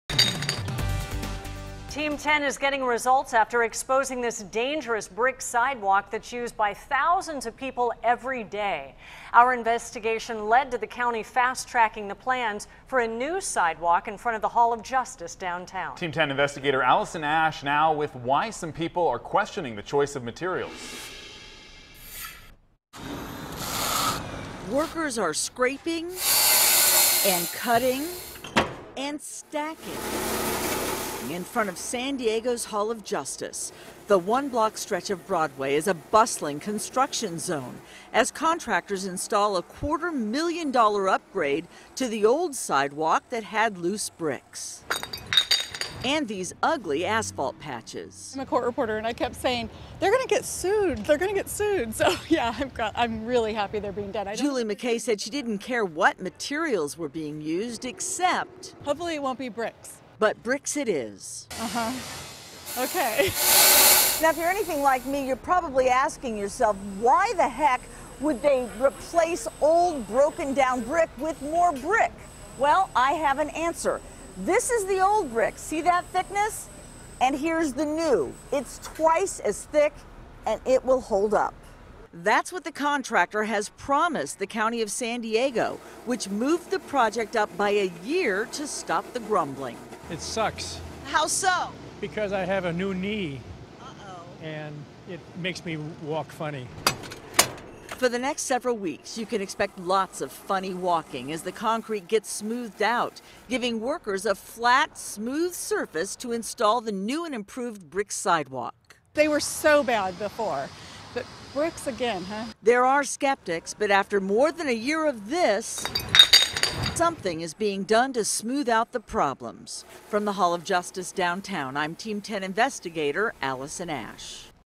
Sidewalk replacement fast-tracked after complaints
Safety concerns over broken bricks at Hall of Justice.